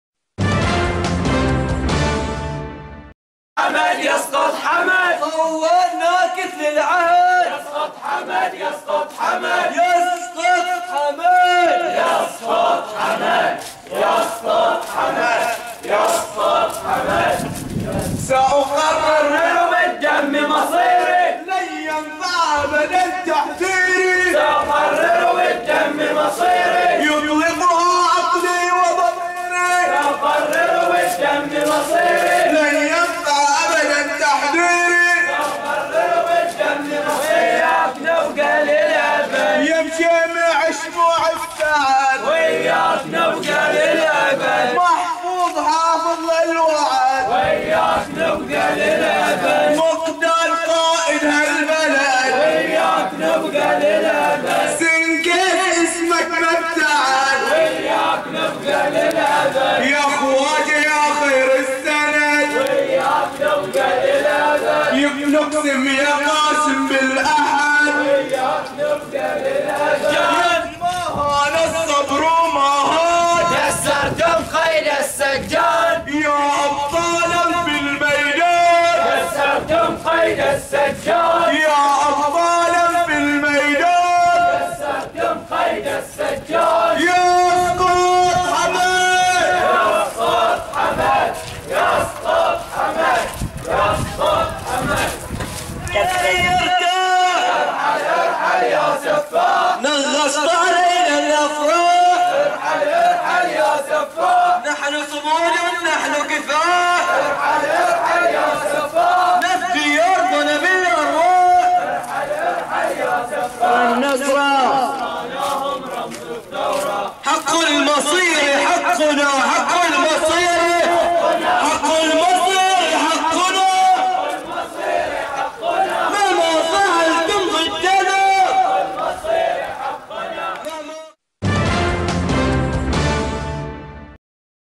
مسيرة "جمعة العزة" استعداداً لخوض عصيان العزة - الحجر 31/1/2014 Bahrain
شارك العشرات في مسيرة "جمعة العزة" التي خرجت في منطقة الحجر غرب العاصمة المنامة اليوم الجمعة (31 يناير/كانون الثاني 2014) تأهباً لخوض الأستحقاق التارخي والعصيان في شهر فبراير المقبل ورفضاً لأي تسويات مع النظام الحاكم في البحرين، وتأكيدا على إسقاطه، وشارك في المسيرة حشود غفيرة حملوا أعلام البحرين ولافتات كتب عليها "لا للحوار مع القتلة"، وردد المتظاهرين شعارات مناهضة للنظام الخليفي وشعارات تدعو للإطاحة بحاكم البحرين حمد بن عيسى آل خليفة .